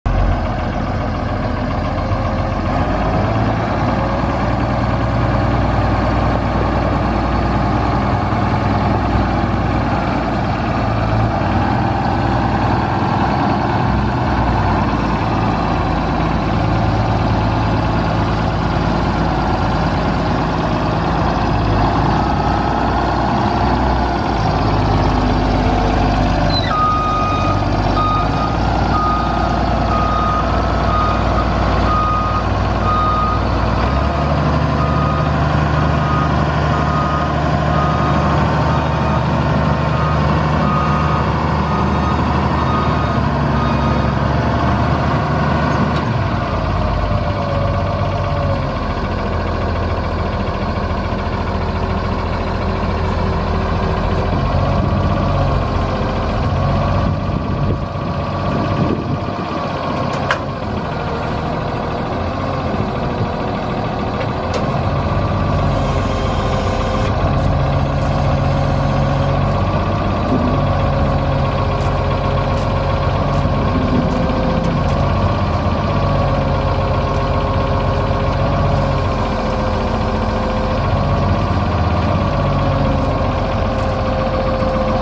1995 Fiat-Allis FG85A Series B motor grader for sale at auction | bidding closes November 27, 2018
1995 Fiat-Allis FG85A Series B motor grader
4,389 hours on meter
2,173 hours on second meter
Cummins 8.3L six cylinder turbo diesel engine
Serial 30429819
Powershift transmission
6F-6R gears
EROPS
AC and heat
AM/FM
Suspension seat
Front and rear work lights
14'W moldboard
Tip control
Side shift
Front mounted lift group
Front mounted scarifier
14R24 tires
Engine issues
Overheats
Window cracked
Serial 71S00581
It's located in Everton, MO, and we're selling it to the highest bidder regardless of price.